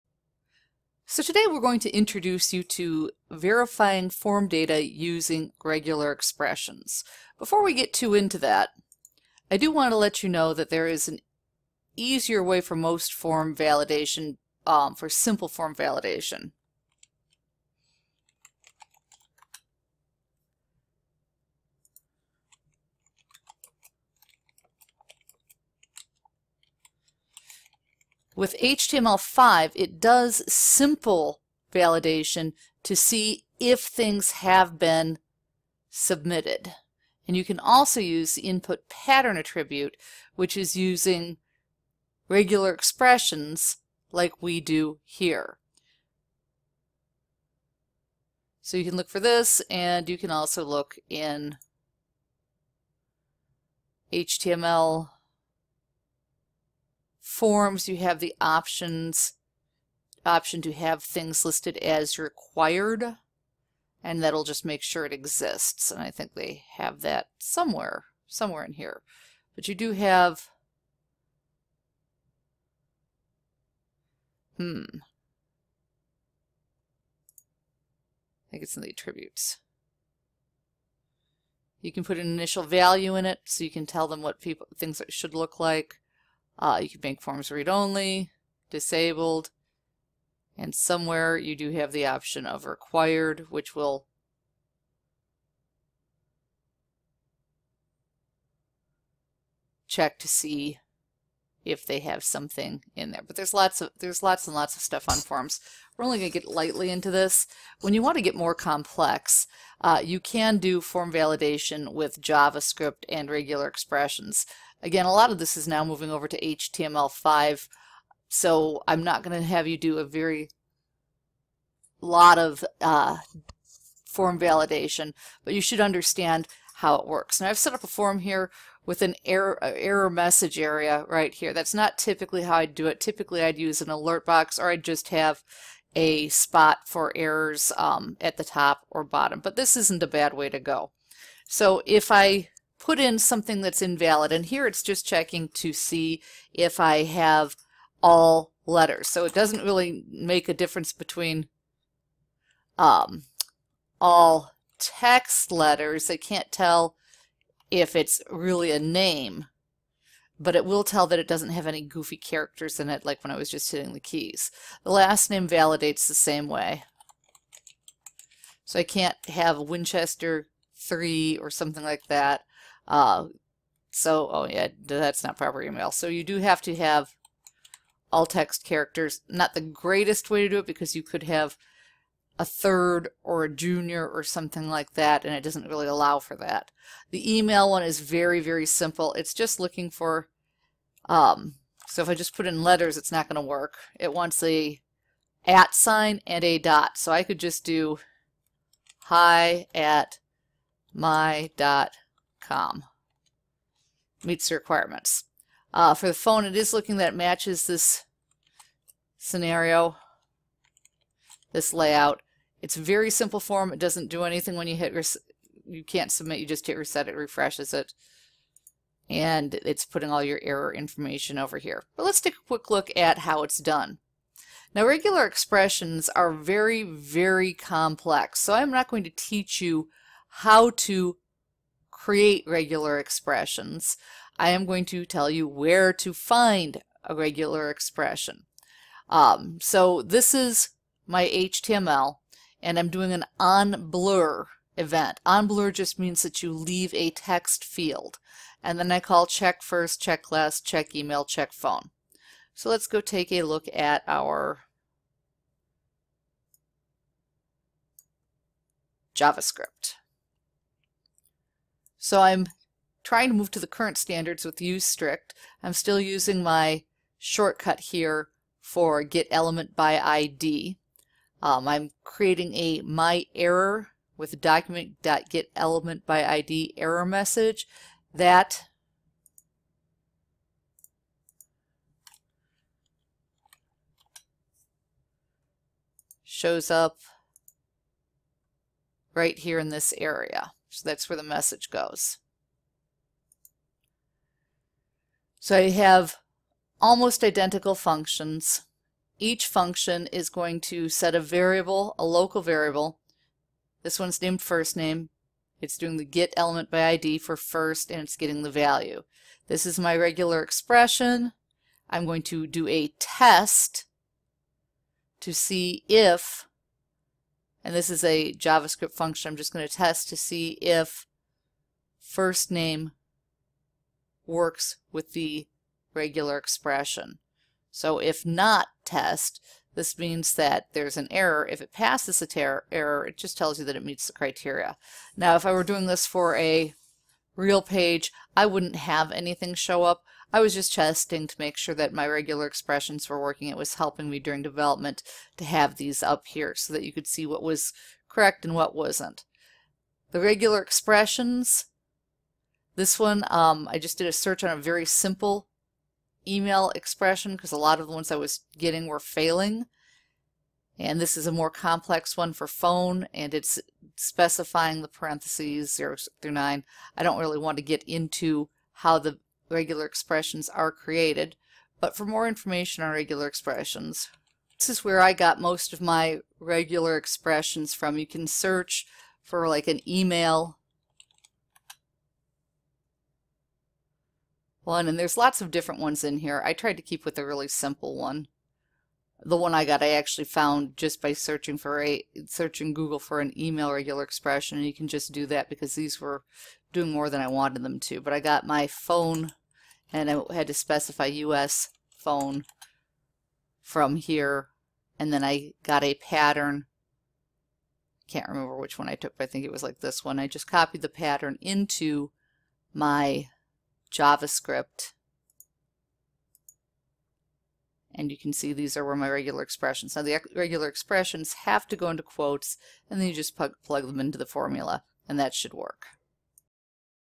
regular expressions in JavaScript
This is about how to use regular expressions, not how to create them